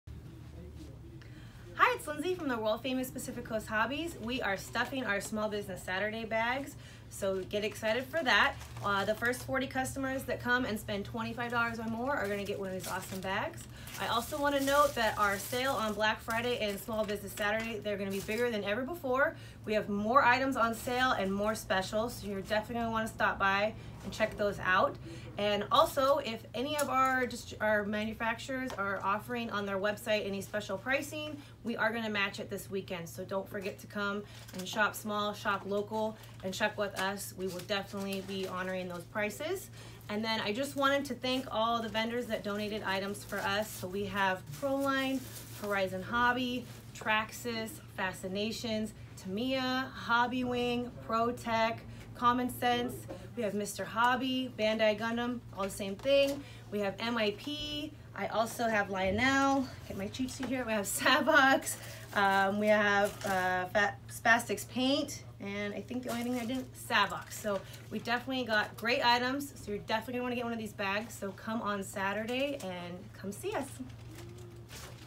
Getting ready for Small Business Saturday
We are getting ready for Small Business Saturday! The first 40 customers who spend $25 or more will get one of these grab bags filled with all these goodies! We are having our biggest Black Friday and Small Business Saturday sale ever! So make sure you stop by and see us. We are also going to honor any of our manufactures Black Friday weekend sales in our store. So shopsmall and shoplocal with us